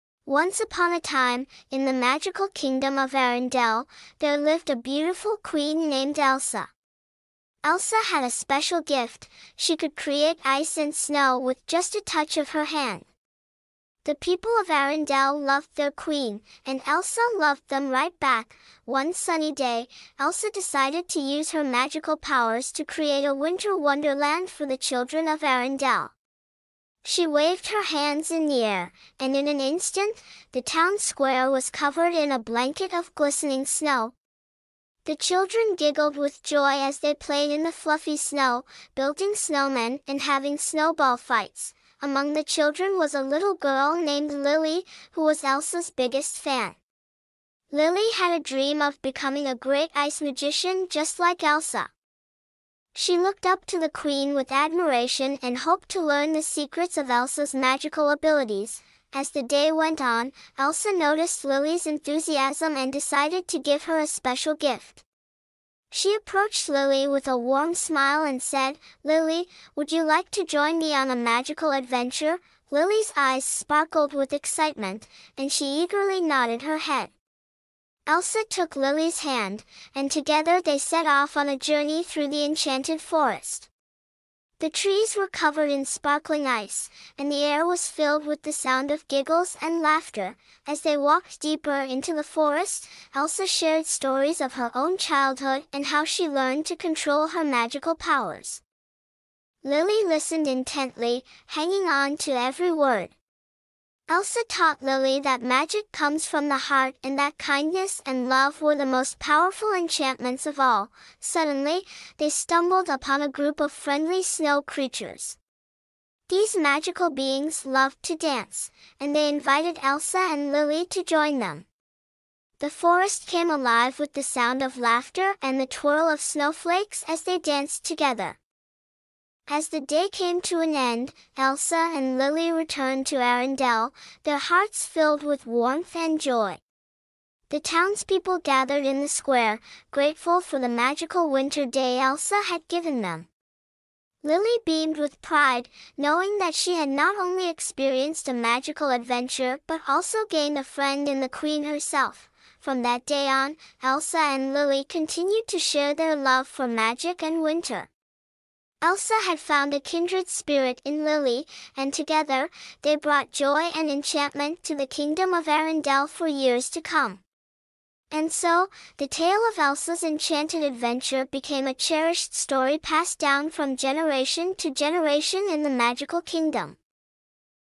Elsa's Enchanted Adventure
Elsa's Enchanted Adventure" is a heartwarming tale set in the magical kingdom of Arendelle, where Queen Elsa, blessed with the power to create ice and snow, decides to gift the town's children with a whimsical winter wonderland. The story unfolds as Elsa befriends a young girl named Lily, who dreams of mastering the art of ice magic. Together, they embark on a magical journey through an enchanted forest, where Elsa imparts valuable lessons about the true essence of magic—rooted in love and kindness. The narrative is a celebration of friendship, as Elsa and Lily share laughter, dance with friendly snow creatures, and bring unparalleled joy to the townspeople. The enchanting adventure becomes a cherished story, resonating with themes of unity, magic, and the enduring power of friendship in the kingdom of Arendelle